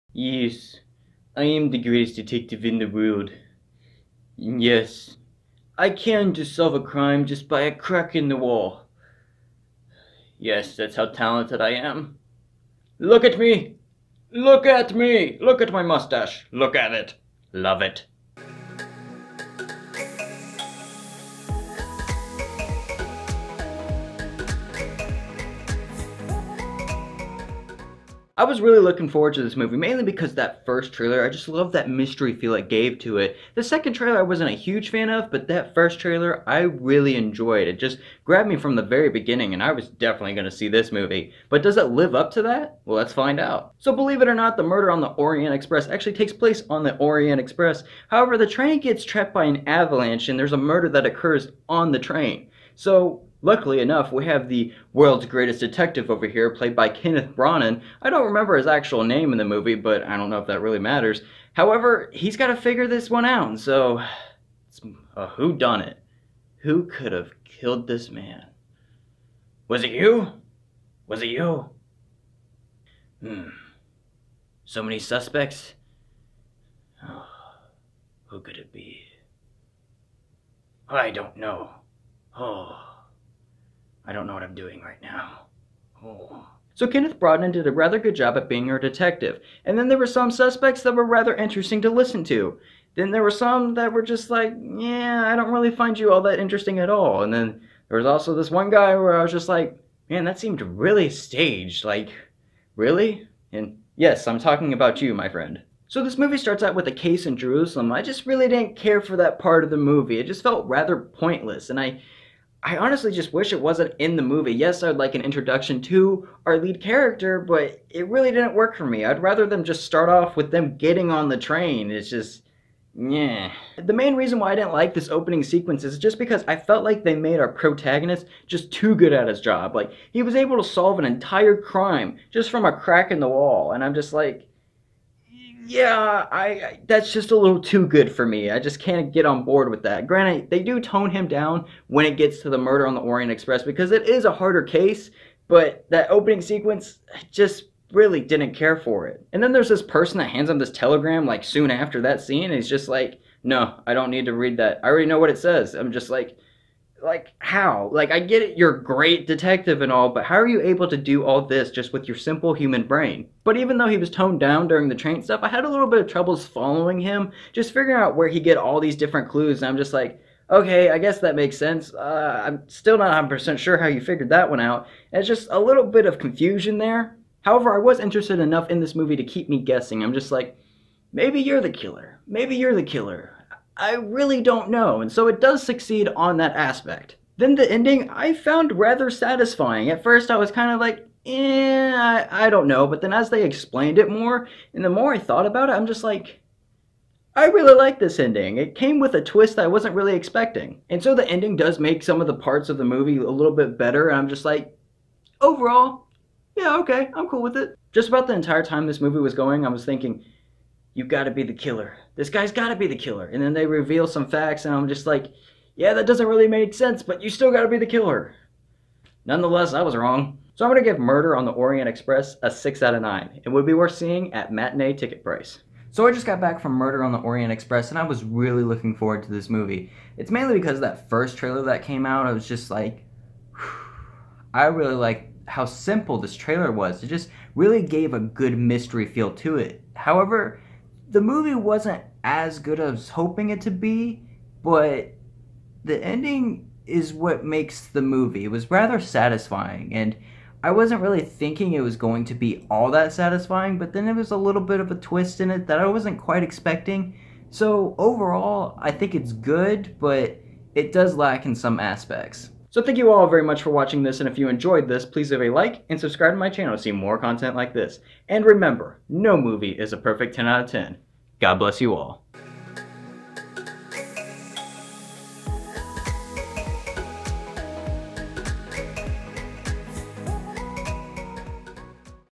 Is Murder On The Orient Express The Murder Mystery We Wanted? - Movie Ticket Review *Spoiler Free
I have not read Murder On The Orient Express, nor have I seen the previous movie adaptation. However, this movie is basically a who done it. Murder On The Orient Express is about this train that gets trapped in this avalanche and someone gets killed on the train. If there is a murder then there is a murderer. So it is up to our world's greatest detective to solve the case and he is played by Kenneth Branagh. Thank you all very much for watching. Remember no movie is a perfect 10/10. God Bless You All.

This is the channel to see spoiler free movie reviews. I try my best to upload each review as quickly as possible.